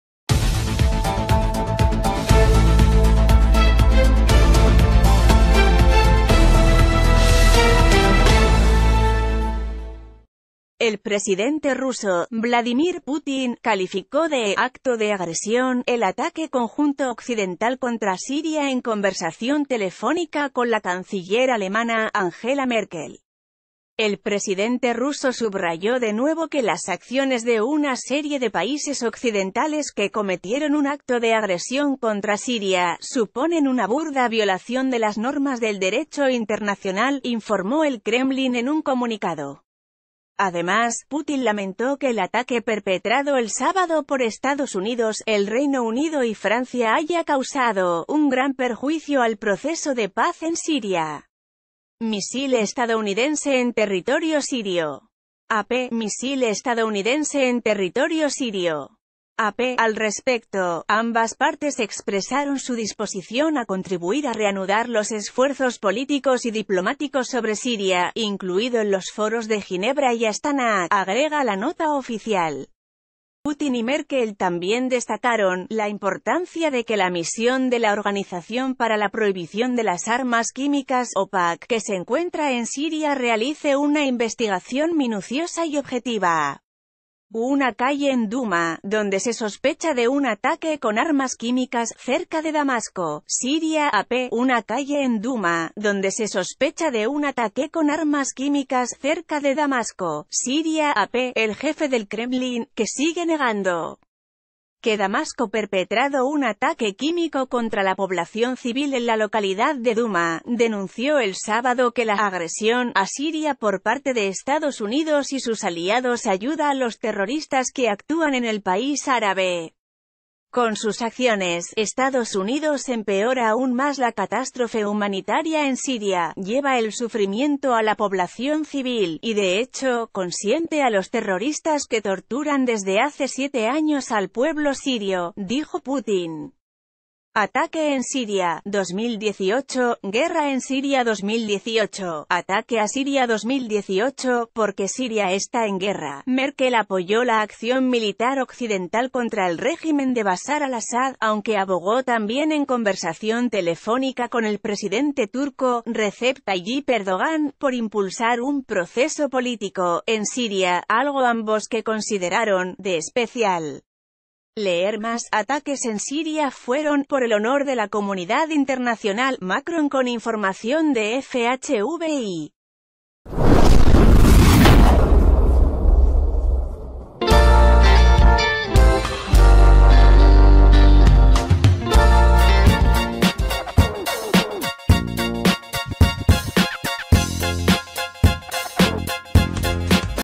Putin dice a Merkel que ataque contra Siria fue un 'acto de agresión'
El presidente ruso, Vladimir Putin, calificó de “acto de agresión” el ataque conjunto occidental contra Siria en conversación telefónica con la canciller alemana, Angela Merkel. “El presidente ruso subrayó de nuevo que las acciones de una serie de países occidentales, que cometieron un acto de agres...